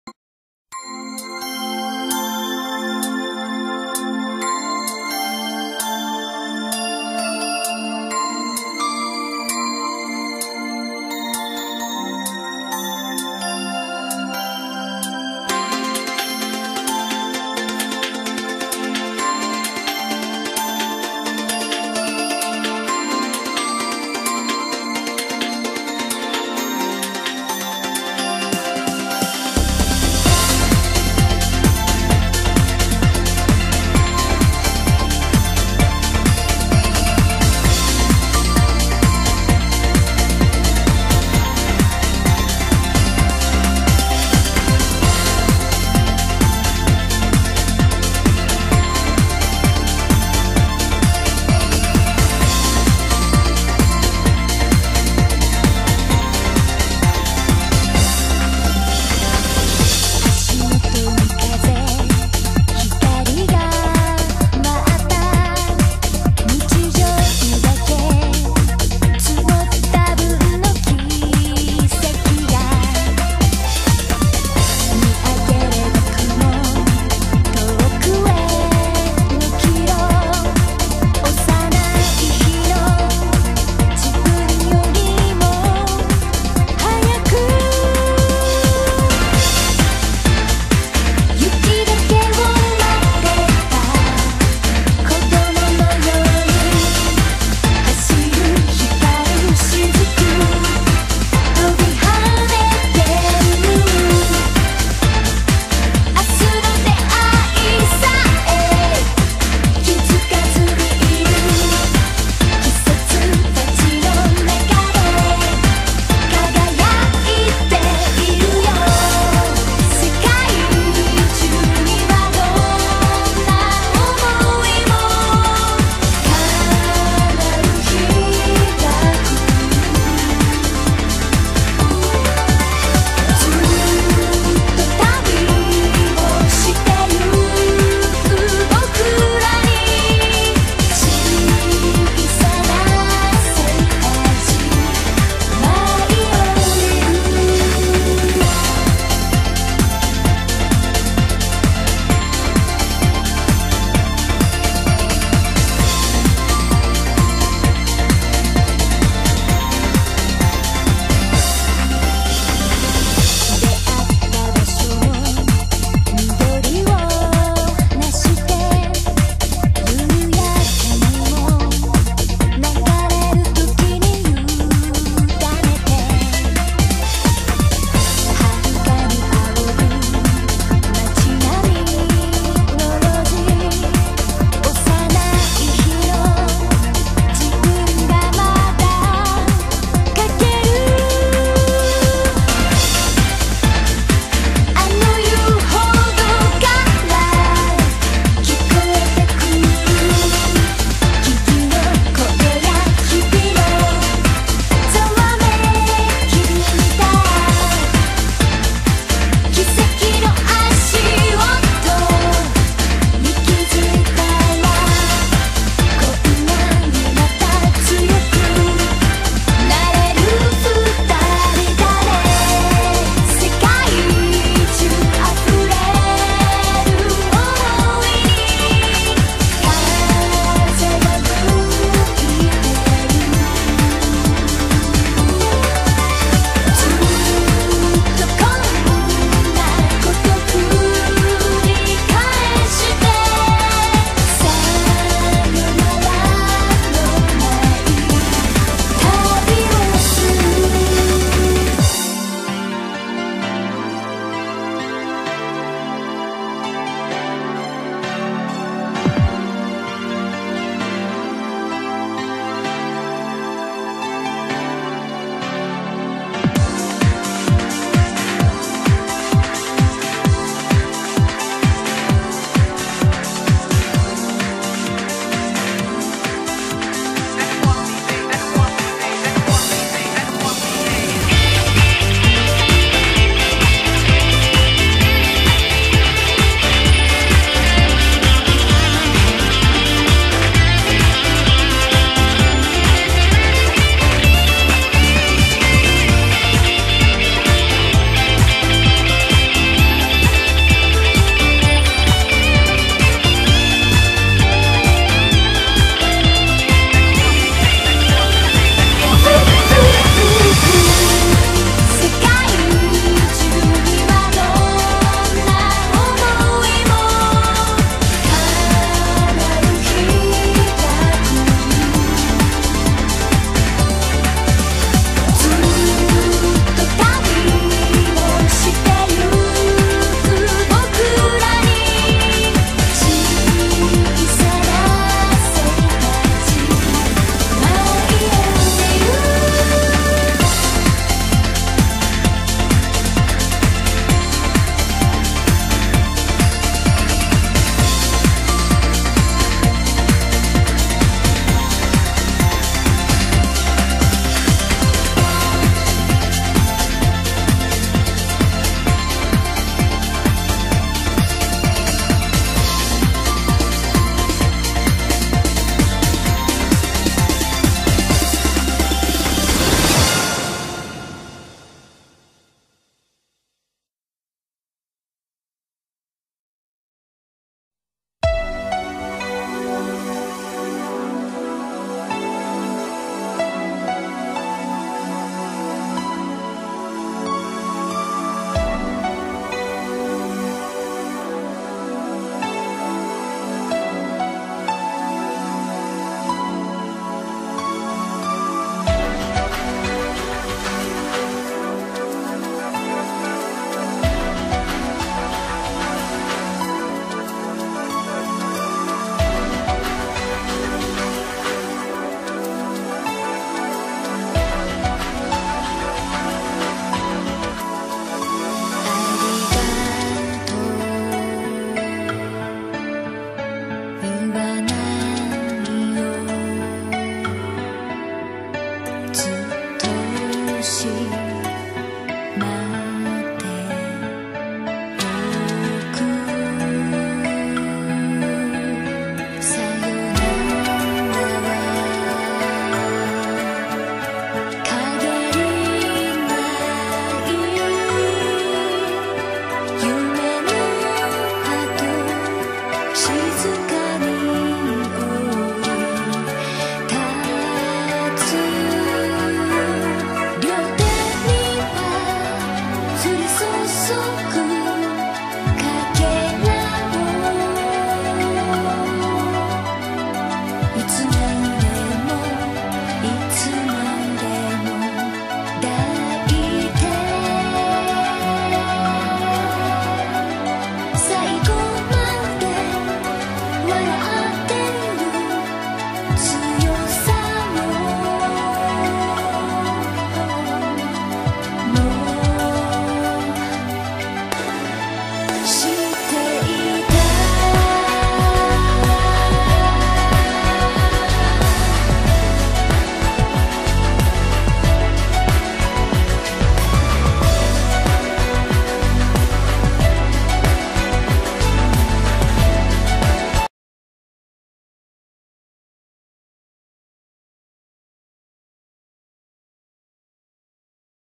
Dibujo de Ayu Tsukimiya (Kanon)
el vídeo de esta semana es el Dibujo de Ayu Tsukimiya (Kanon) después de casi arreglar todo para que la edición salga bien si loes gusta denle me gusta y/o suscribanse al canal tratare de traer mas dibujos

(este dibujo es para Mauricio Perez por haber adivinado la adivinanza de los opening que puse en face espero te guste)